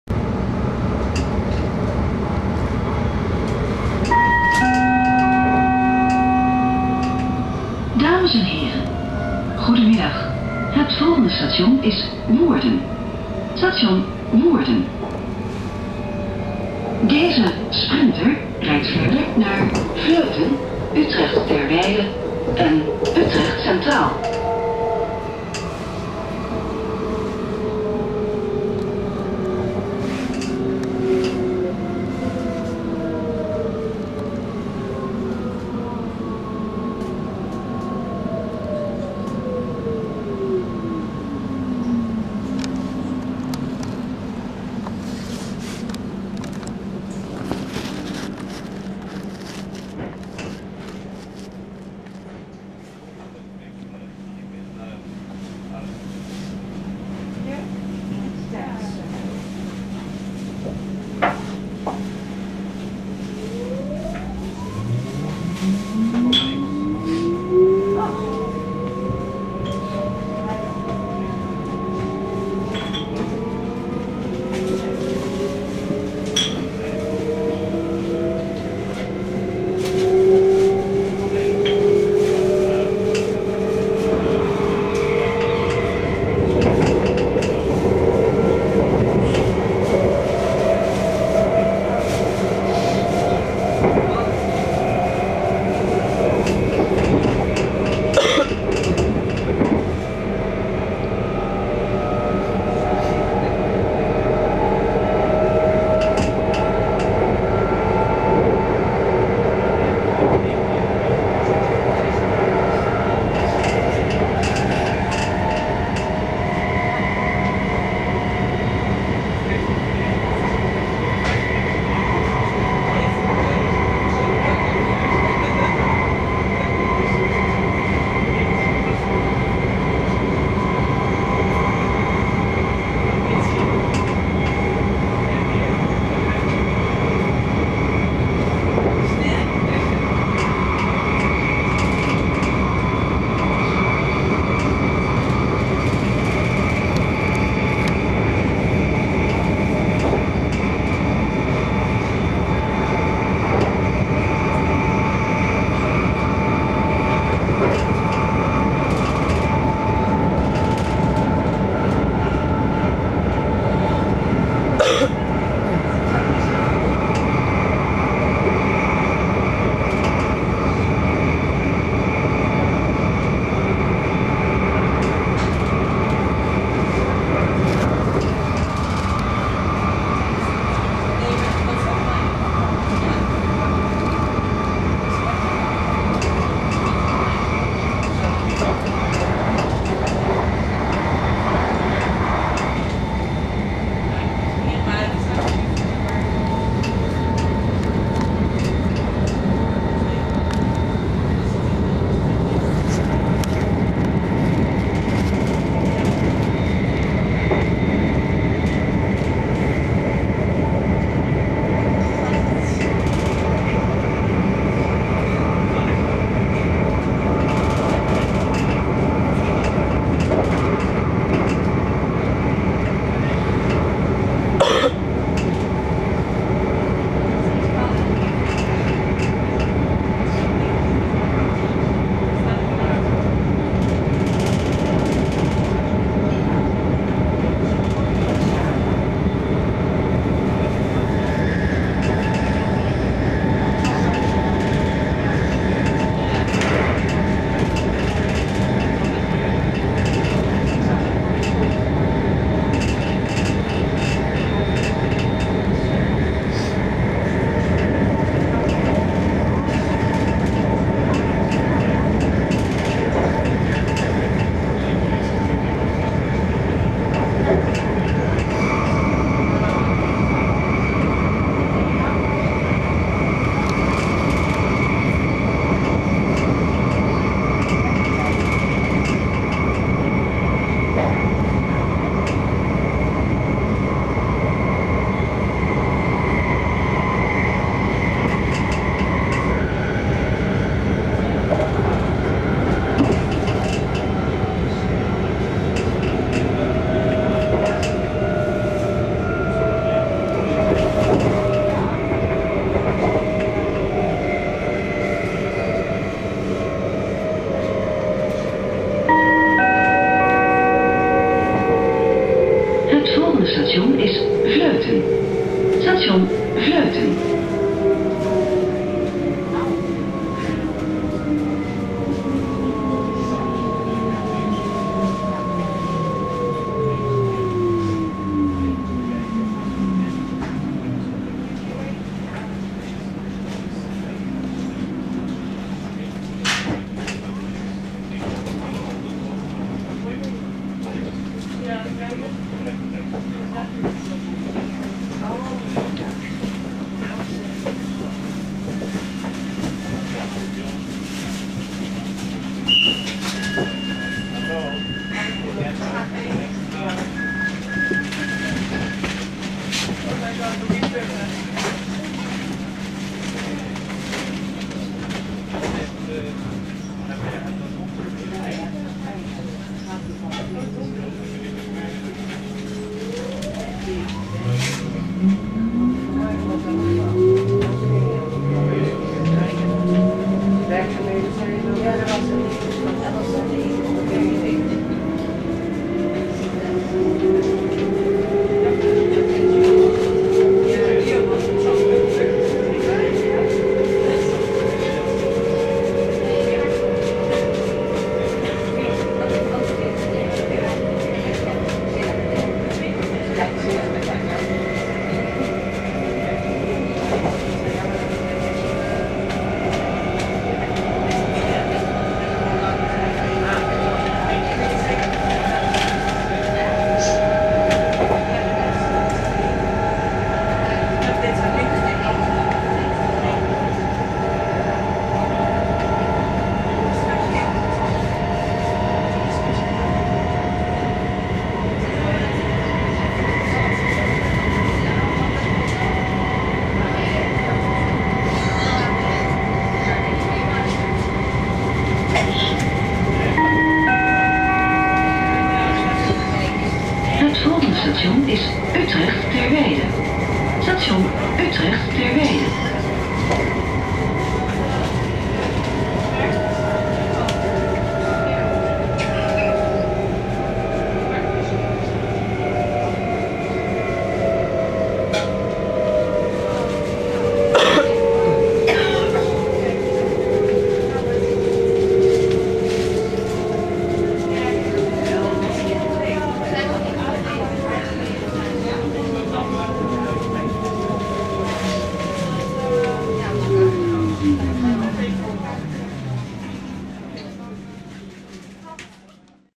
Van Woerden naar Utrecht Terwijde met SLT 2412
26 maart 2011.
Een ritje met de 2412 van station Woerden tot station Utrecht Terwijde.